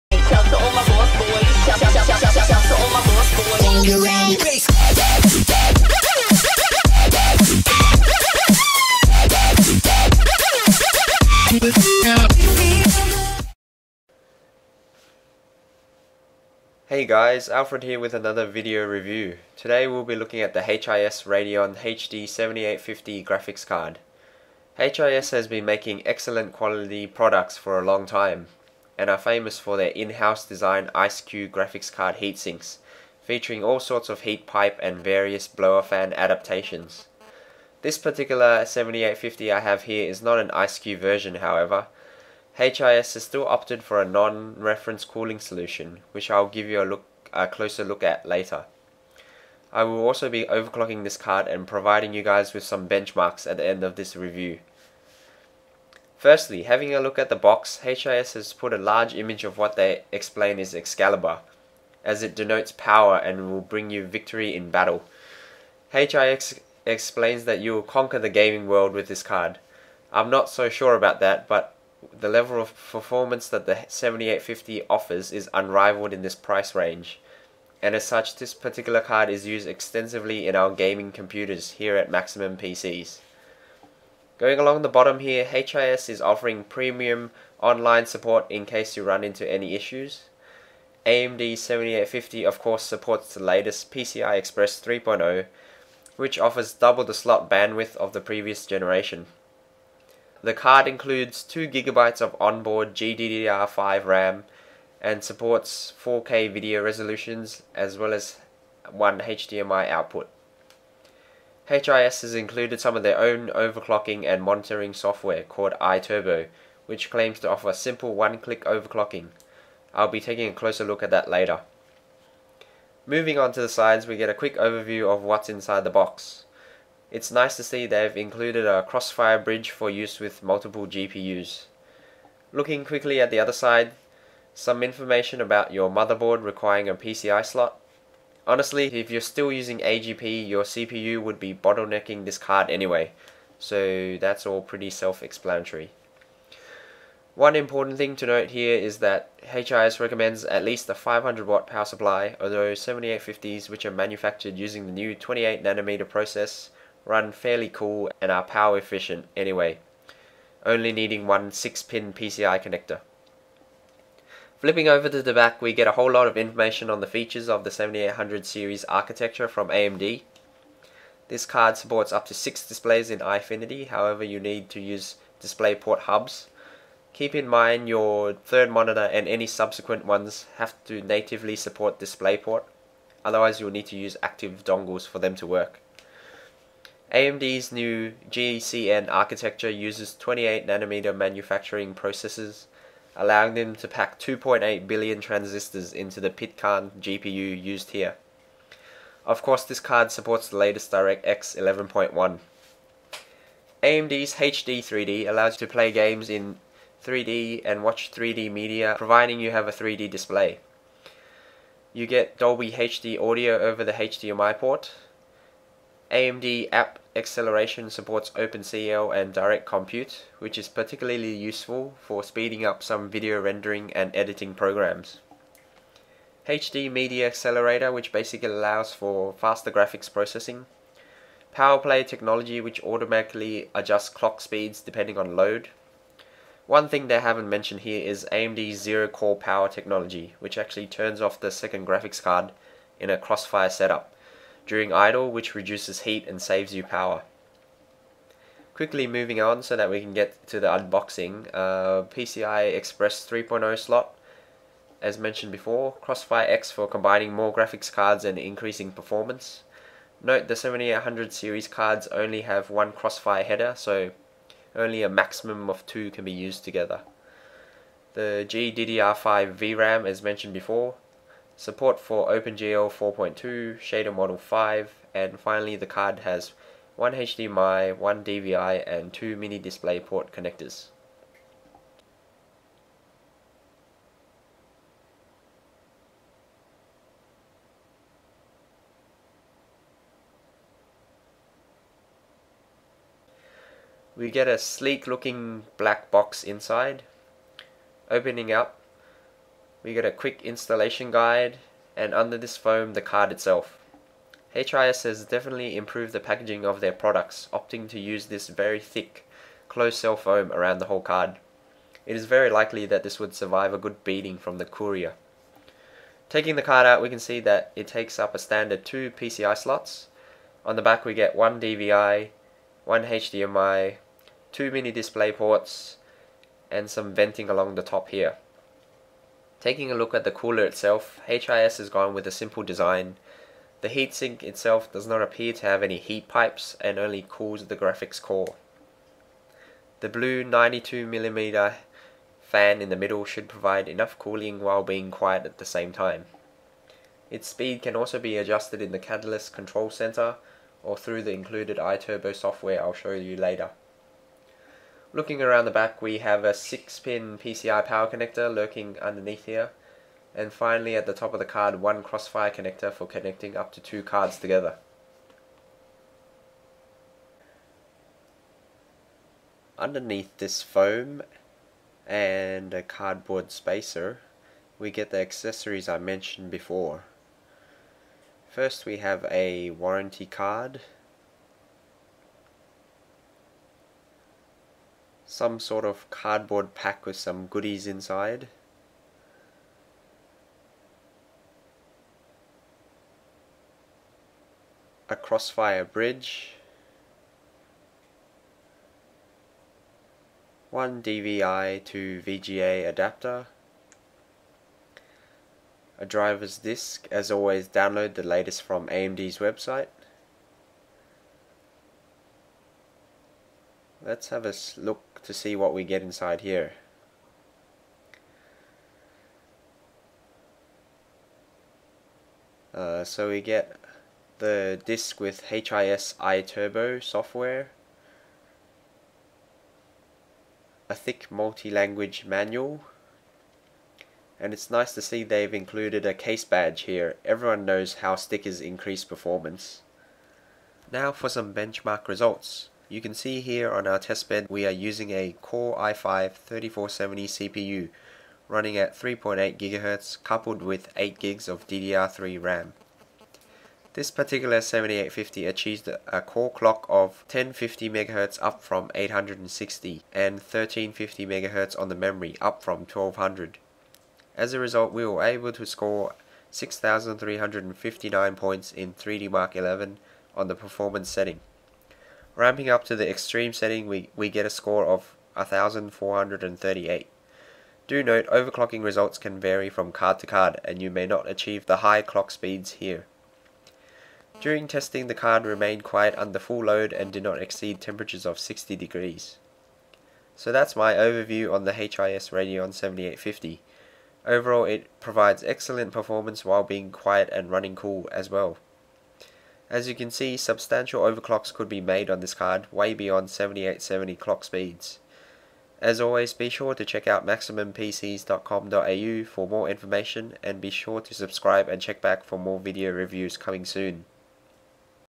HIS Radeon HD7850 graphics card unboxing and review - Maximum PCs Australia
The HIS 7850 represents excellent value performing extremely well and being able to overclock high as well all while running cool & quiet. For these reasons we use these cards extensively on our high performance computers.

- Customised performance computing NO compromises!

OC achieved: core 1050 Mhz, memory 1350 Mhz
3DMark 11 scores: P6359 and X1438